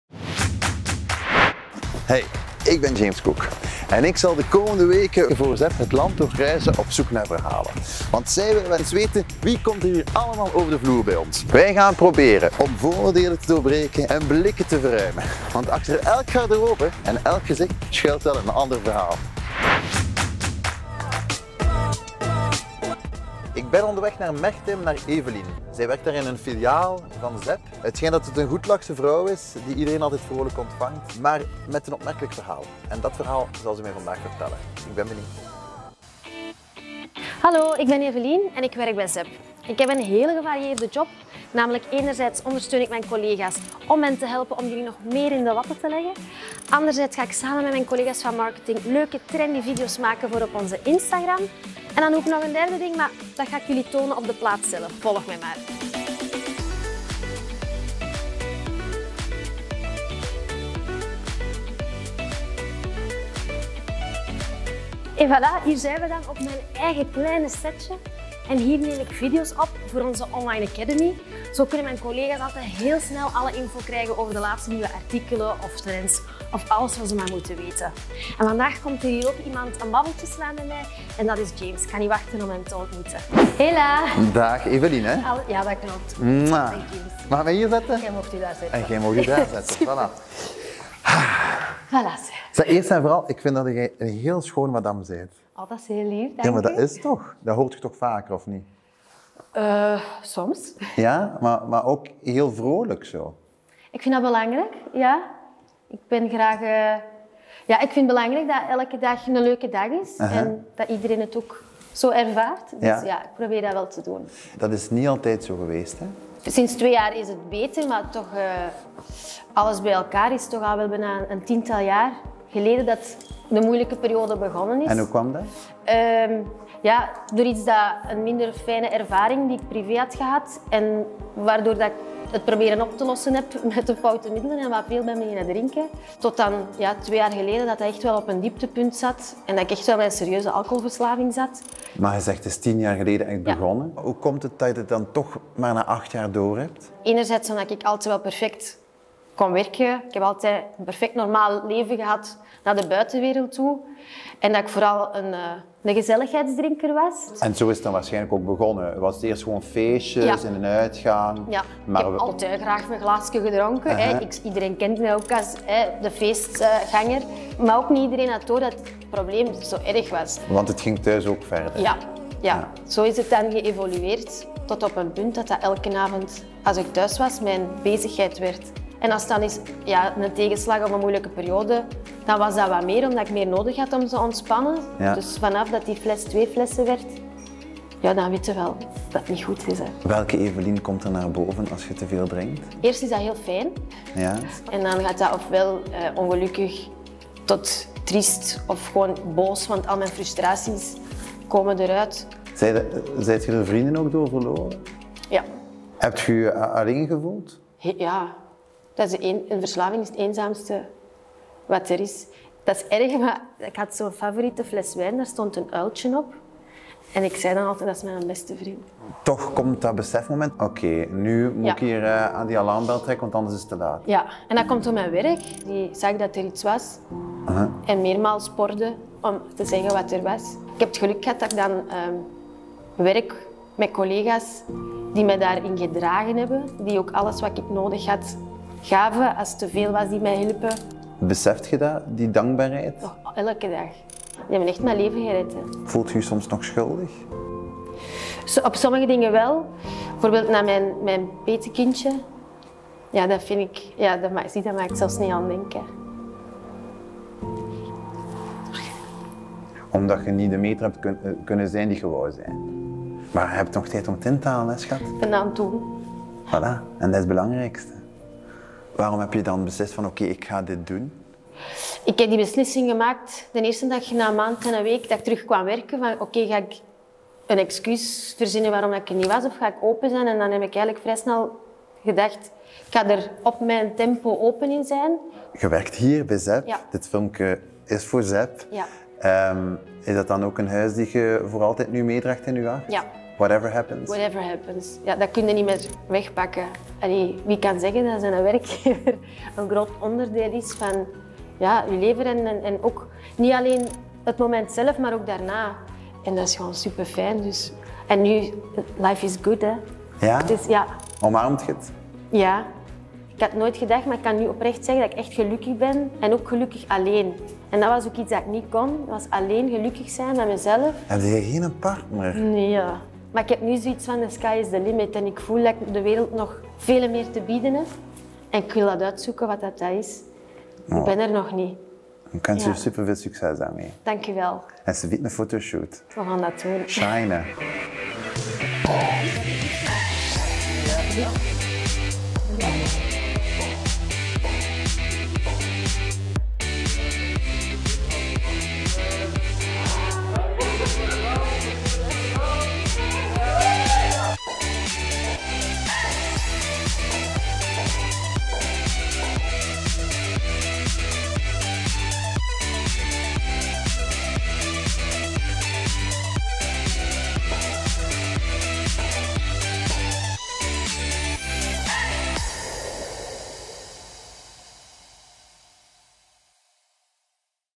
ZEBFOREVERYONE: Evelien vond zichzelf terug na haar alcoholverslaving
James Cooke gaat op bezoek bij de altijd goedlachse Evelien, die vertelt over haar alcoholverslaving en hoe ze die overwon.